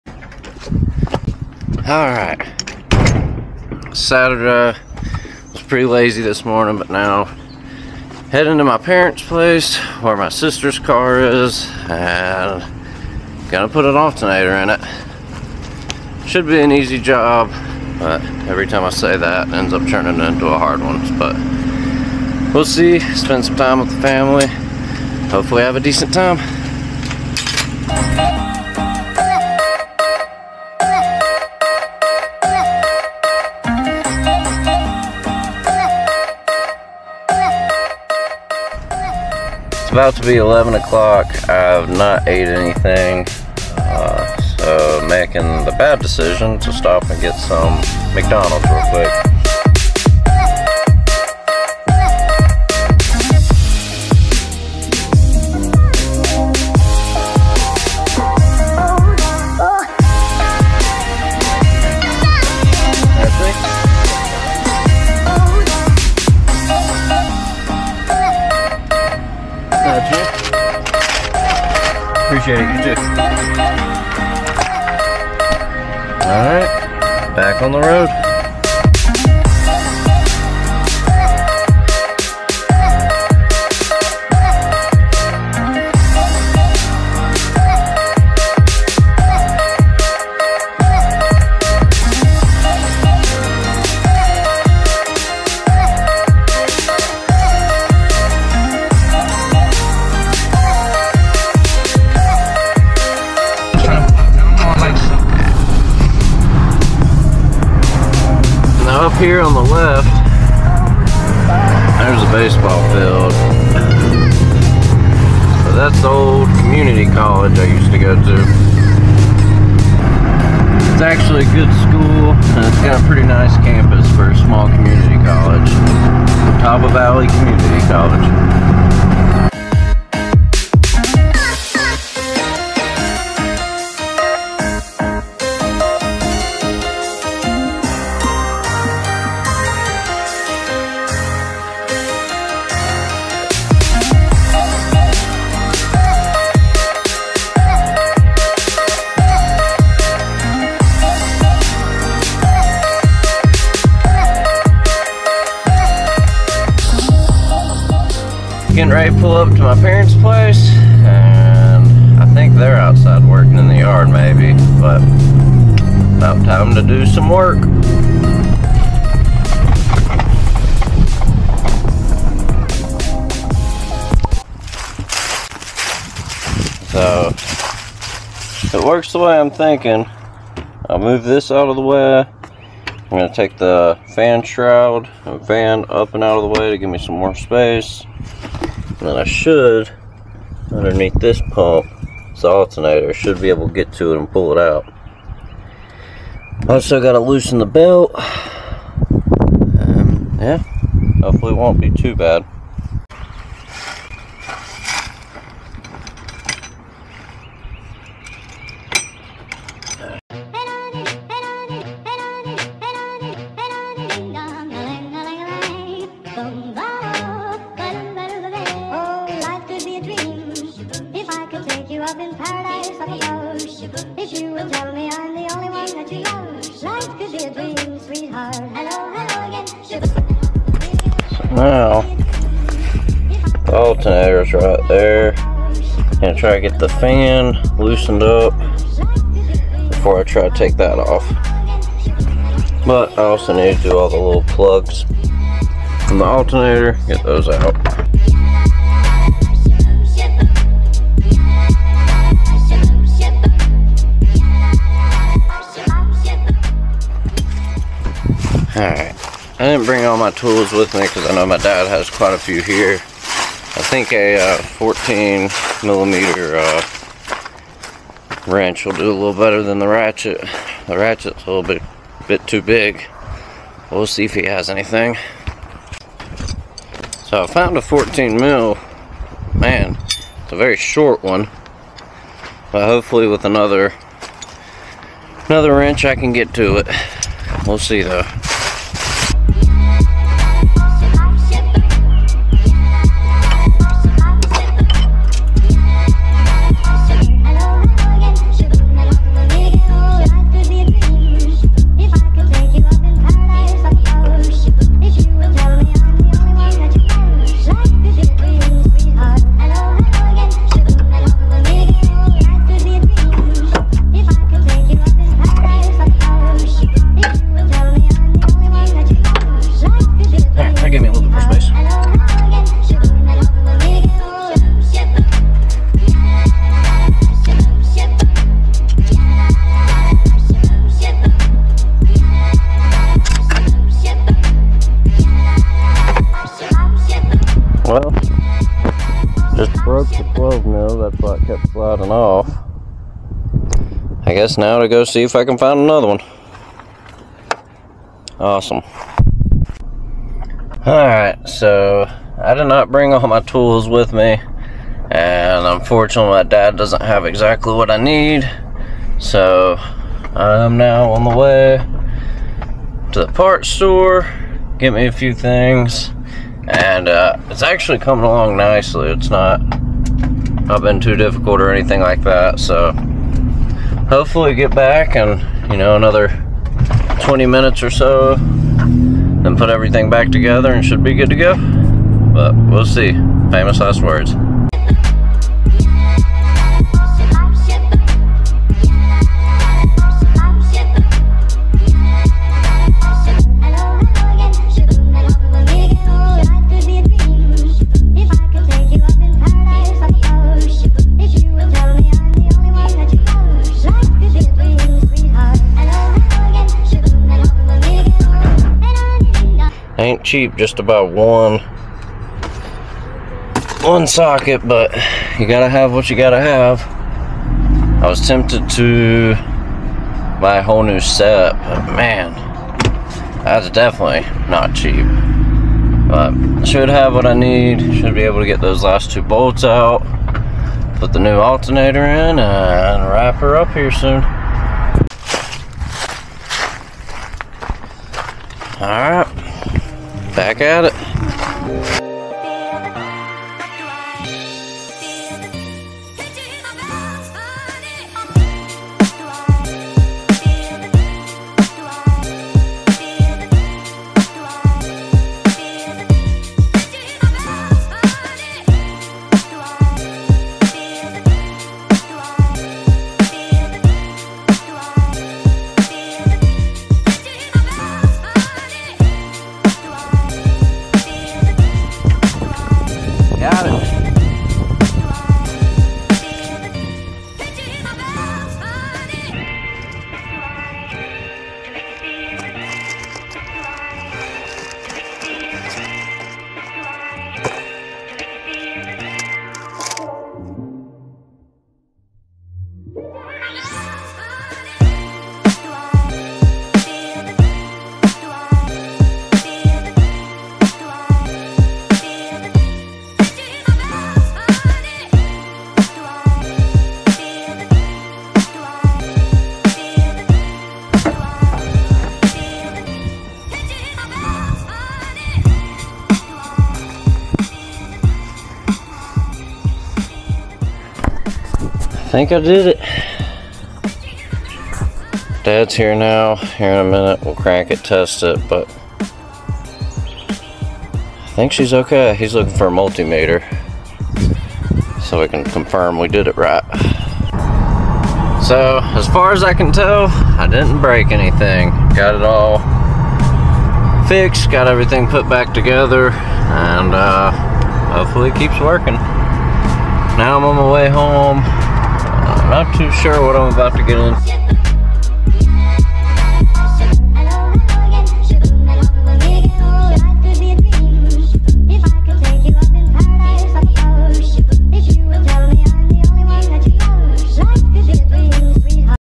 08 Acura RDX alternator
Helping fix a car problem my sister was dealing with.